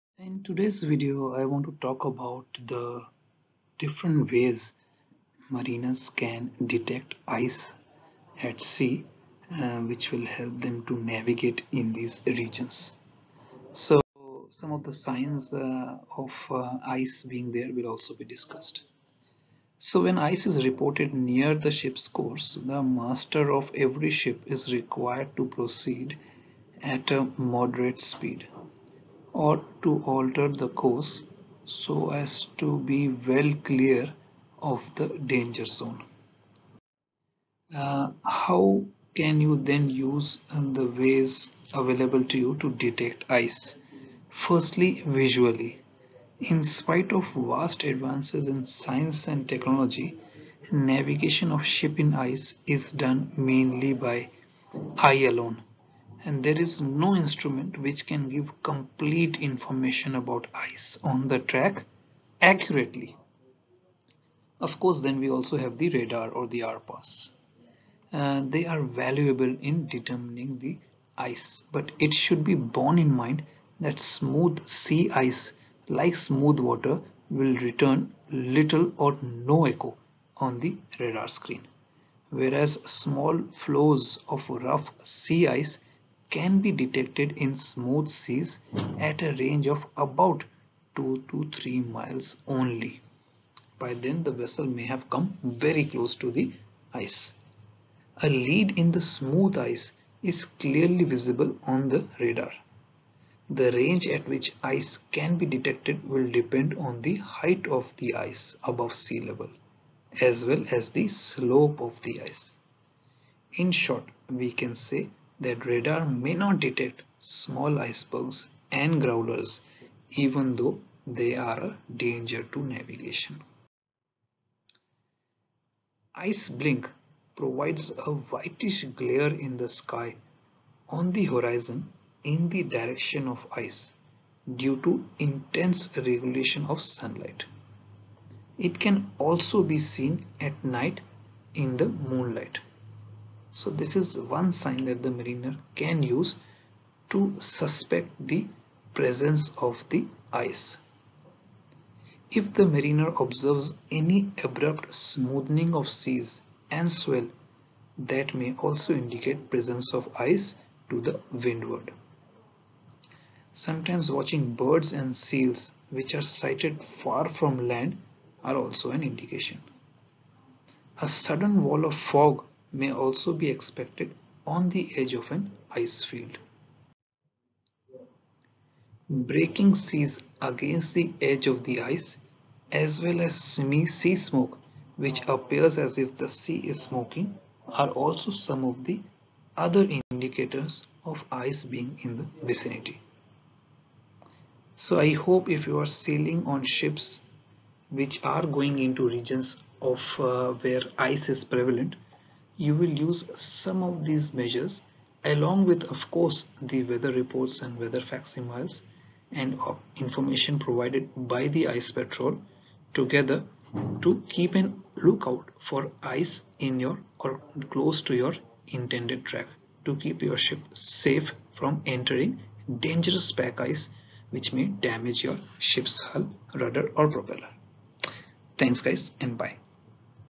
Detection of ice in high latitudes by mariners
This video discusses the different ways mariners can detect the presence of ice at sea.
Students and mariners must watch other videos listed below to enhance knowledge of the topic: